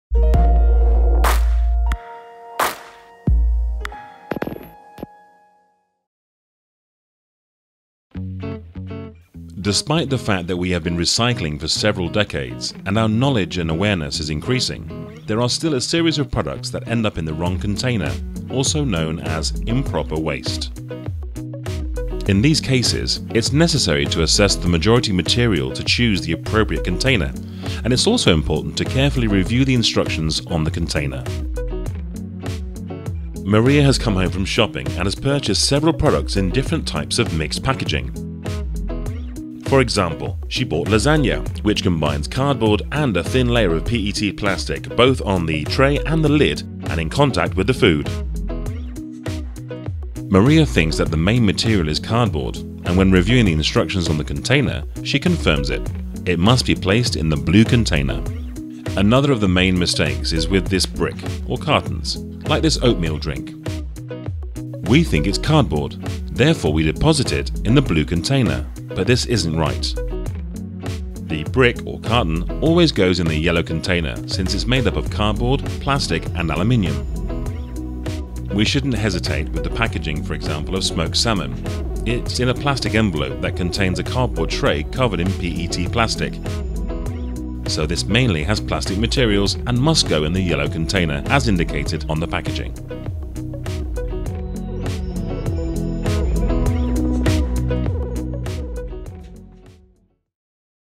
Avoiding Common Recycling Mistakes with Mixed Packaging
Learn how to properly recycle mixed packaging and avoid common recycling errors. It's crucial to read packaging labels carefully to determine the correct recycling container, as different parts of a package may belong in different bins. Improper waste disposal often occurs when mixed packaging ends up in the wrong containers. To prevent this, assess the primary material of the packaging and follow the instructions provided. These steps help reduce recycling mistakes and contribute to a more efficient recycling process.